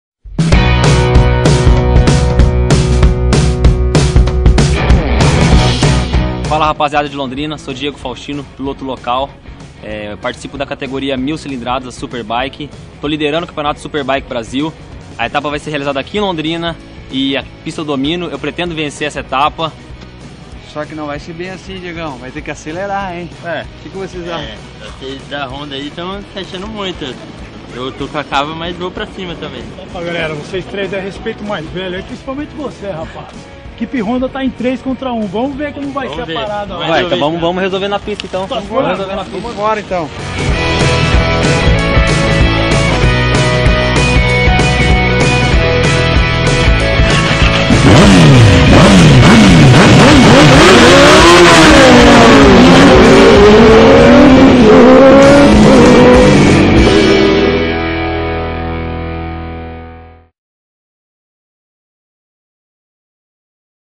SBK 2015 - 3ª etapa SuperBike Brasil Londrina!
Os pilotos da categoria SuperBike prometem que a disputa vai ser dura nesta 3ª etapa do campeonato. Você não pode perder!
Vemtodomundo. 26 de Julho Autódromo Internacional Ayrton Senna.
Video: Marcos M. Carmona, Guto Piercing e Eduardo Azeredo/ VGCOM
Edição: Neto Barros/ VGCom